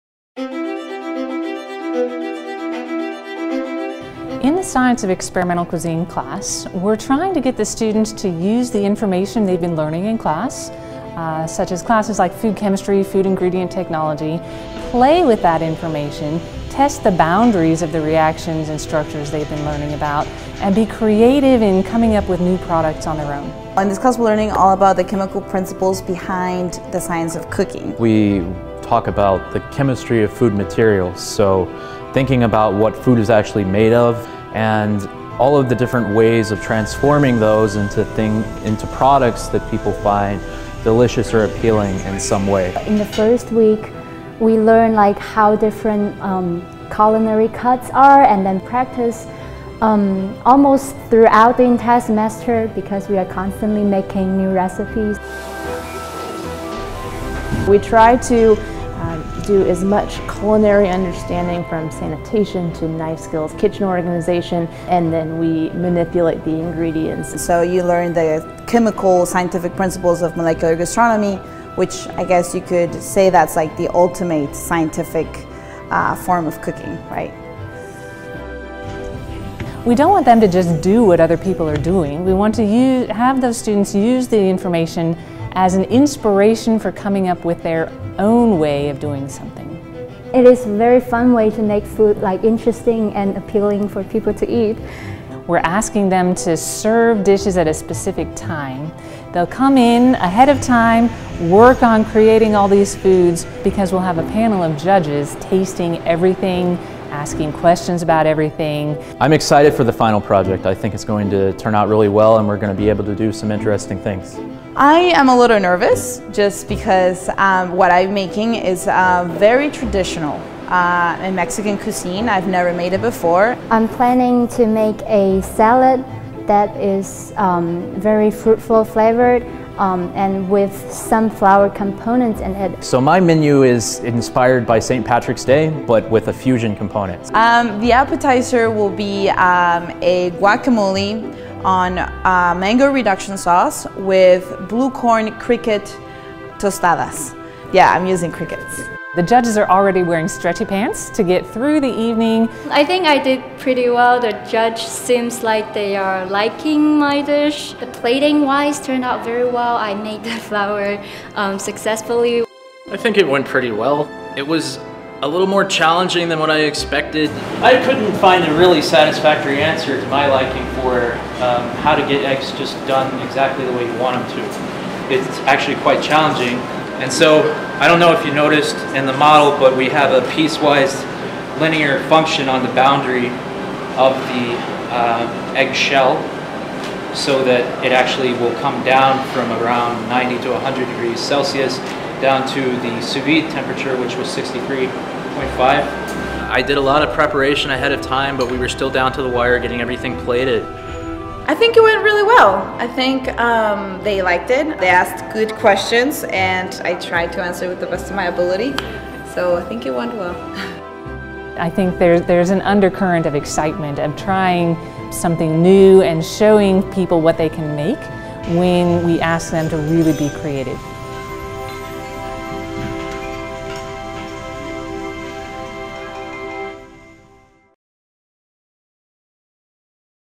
The Science of Experimental Cuisine
Purdue University’s Science of Experimental Cuisine course in the Department of Food Science dives deep into the application of food chemistry, analyzing the molecular capabilities of ingredients and how they can be manipulated. Food Science students are trained extensively in food chemistry, processing techniques and product development, and this course gives them insights into how products they might work with will be used by chefs and home cooks.